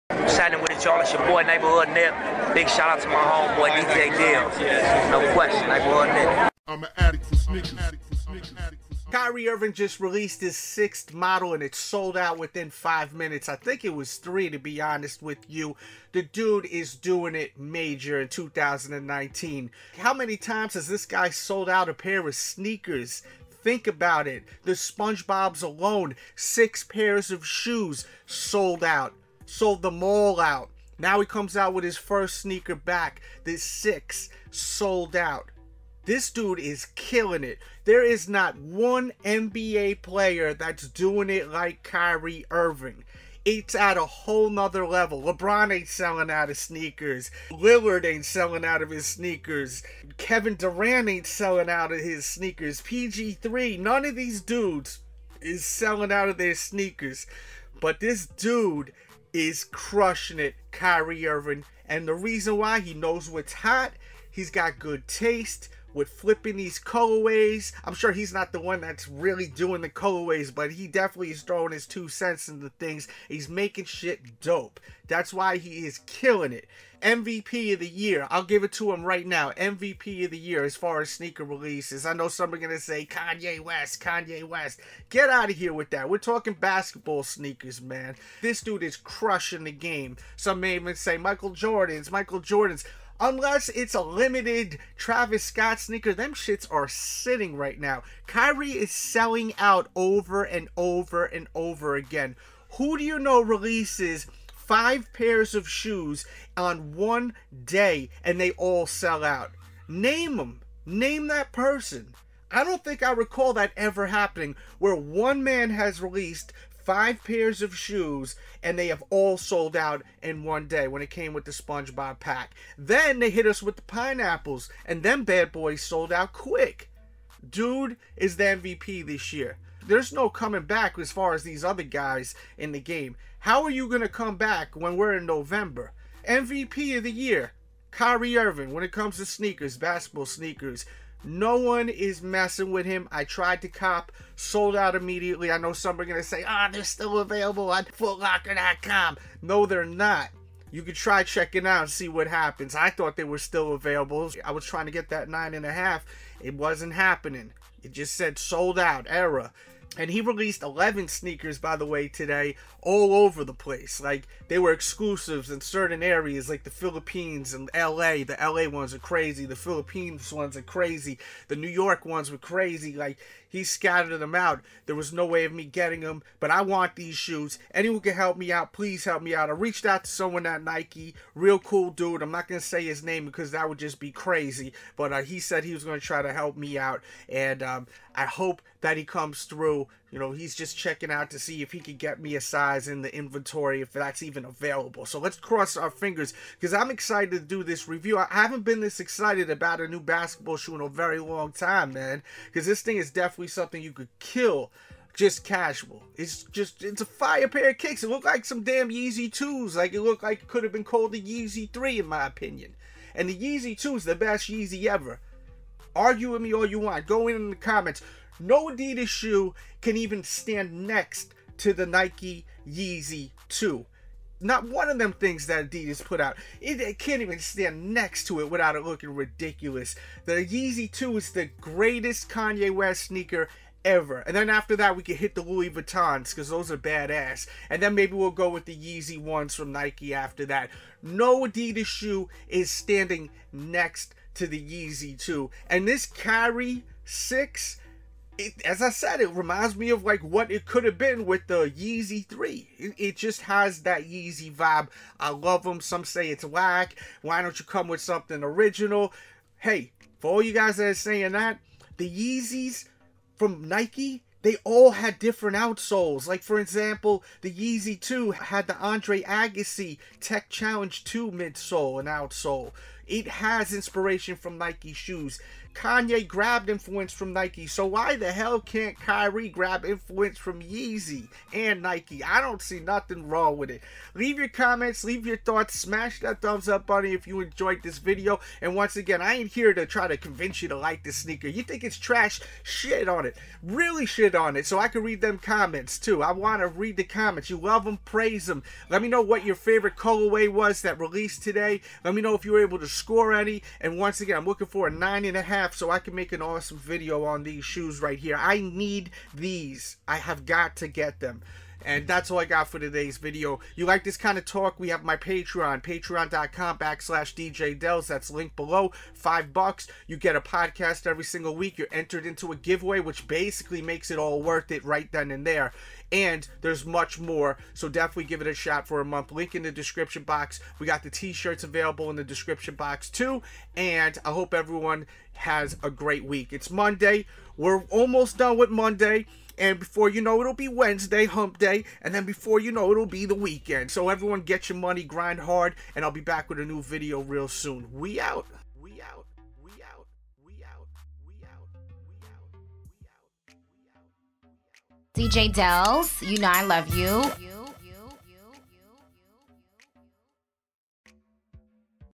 NIKE KYRIE 6 SNEAKER SELLS OUT IN MINUTES, KYRIE IRVING THE MVP OF SNEAKERS THIS YEAR
Nike Joggers

Camera's I use on youtube:
Canon g7x Camera
Panasonic G7 Camera